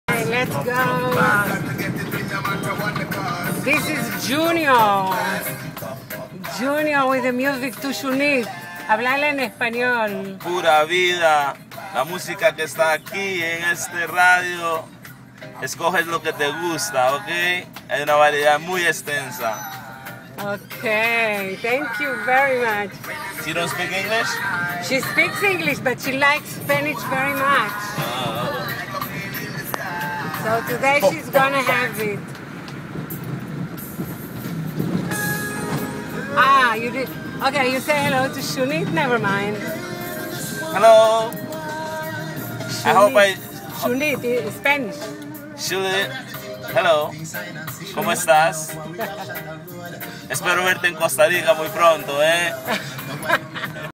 Junior in costa rica - casa galim
טיול מעולם אחר אל היער האינדיאנים בקוסטה ריקה -